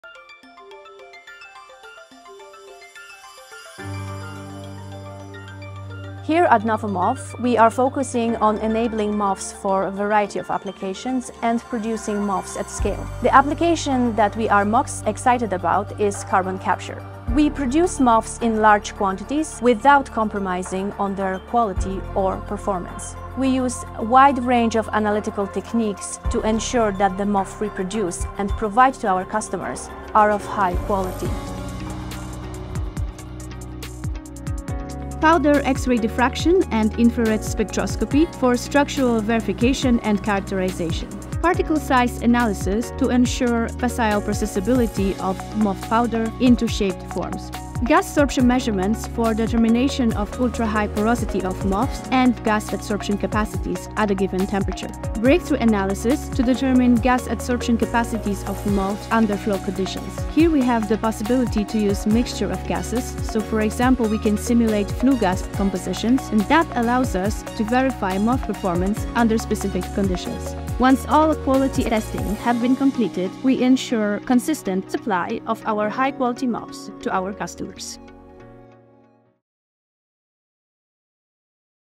Scaling MOFs for carbon capture
Mastering MOF production for CarbonCapture with advanced analytical techniques: particle size analysis, gas sorption measurements, breakthrough analysis, powder X-ray diffraction and infrared spectroscopy. Ready to cut your CarbonFootprint?